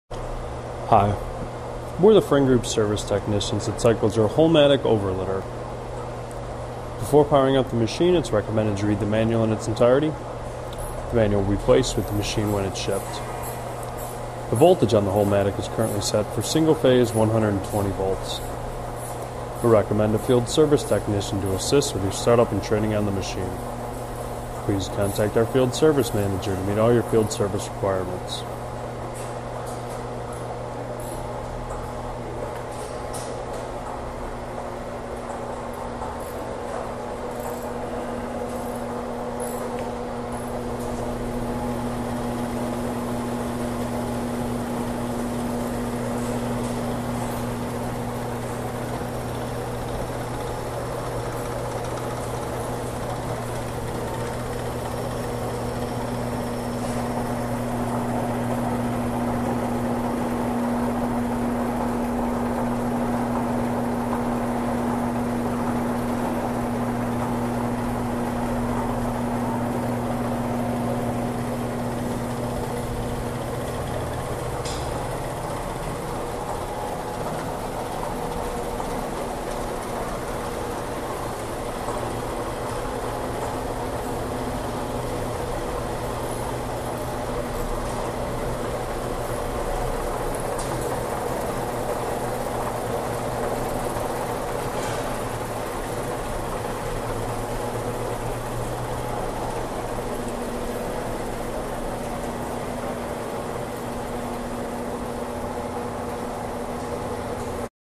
Holmatic Automatic, stainless steel, inline overlidder
Automatic, stainless steel, inline overlidder rated from 60 to 150 lids per minute - speeds depending on materials, application and machine configuration. Equipped with a 4.5" wide x 164" long Delrin conveyor with adjustable guide rails, star wheel, 48" long spinning rod lid magazine, snap on lid applicator, top compression section, no container no lid, variable speed controls and interlocking Lexan safety guarding. Mounted on height adjustable legs.